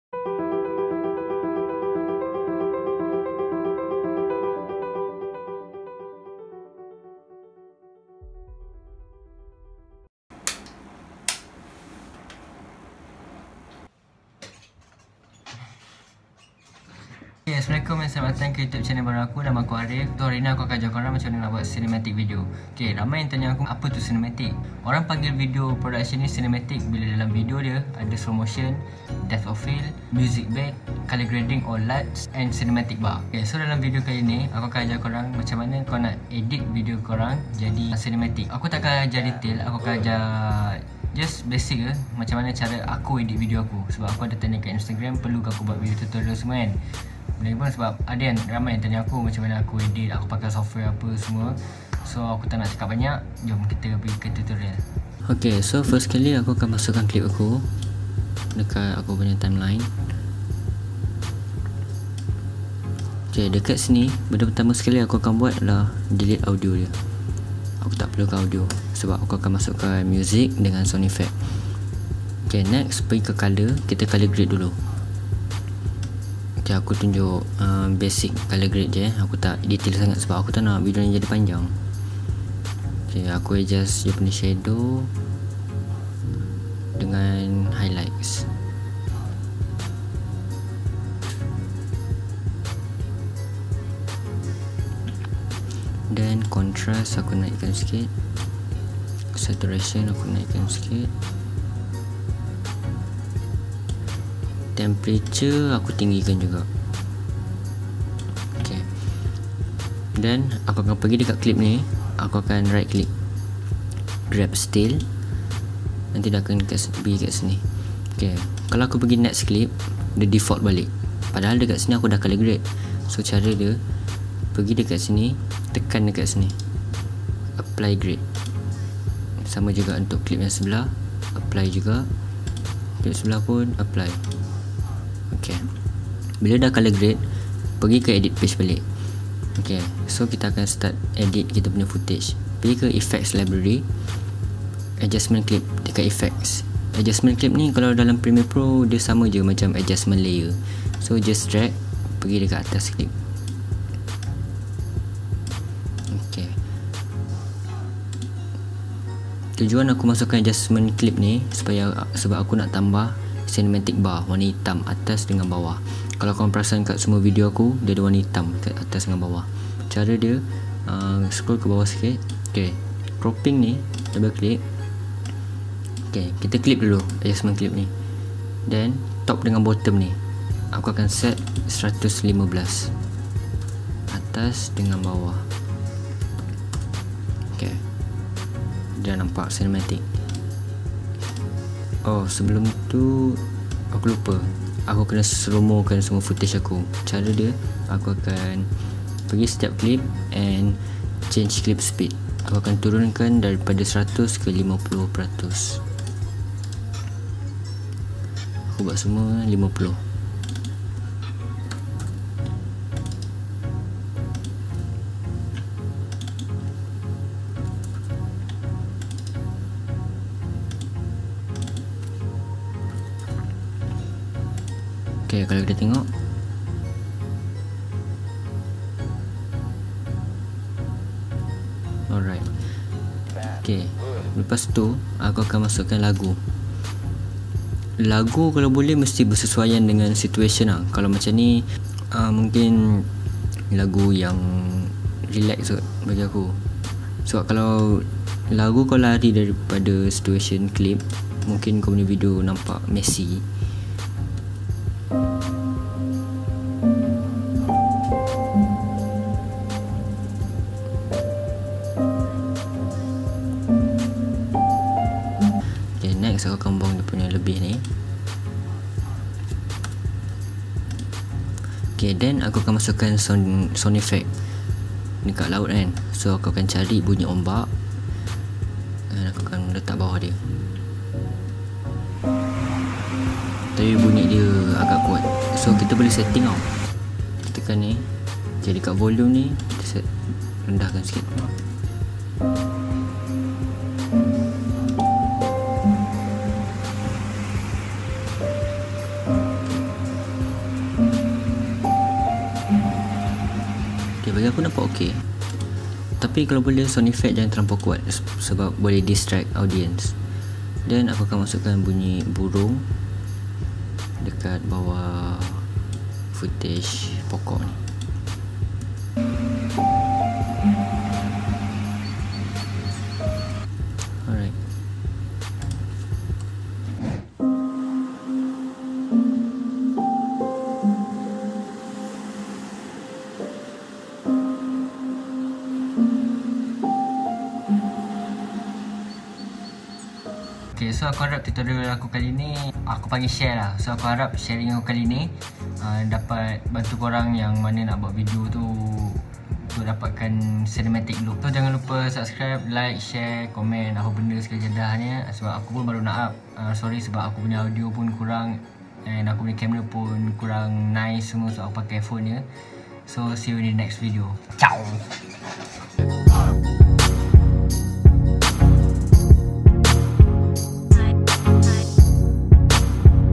CINEMATIC LOOK || Davinci Resolve
Baru-baru ni aku ada tanya dekat Instagram 'perlu ke aku buat tutorial CARA AKU edit video' dan ramai yg vote YES..so harini aku decided untuk buat video tutorial since ramai yang tanya aku cara aku edit..I'm sorry sebab audio dengan quality camera kurang sikit sebab aku record pakai phone